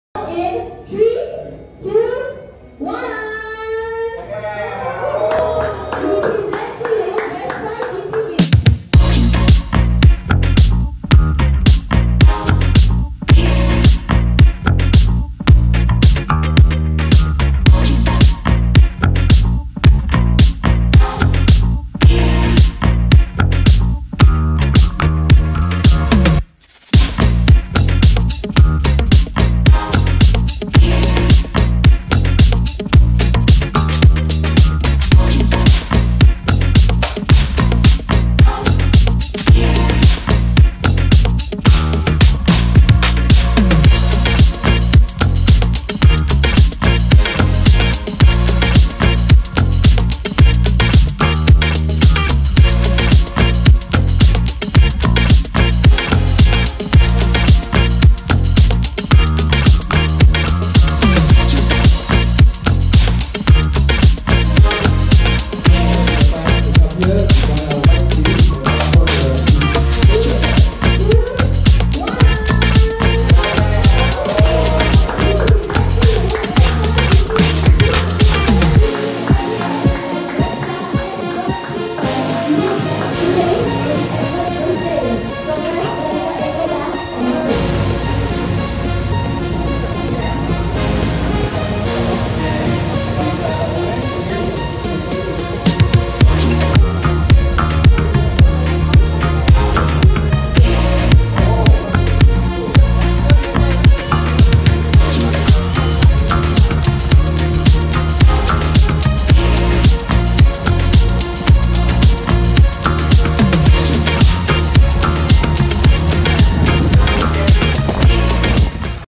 🛵🎉 Unveiling of the 2023 Vespa GTV 300 HPE ABS! 🚀
On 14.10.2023, we set the wheels in motion with a spectacular launch of the all-new 2023 Vespa GTV 300 HPE ABS. It was a day filled with excitement and style as we welcomed this iconic Italian beauty into our family.

A sincere thank you to everyone who joined us, ensuring that this event will be remembered for years to come. Congratulations to our new owners, and a warm welcome to the Vespa family!

Ready to become a part of the Vespa family?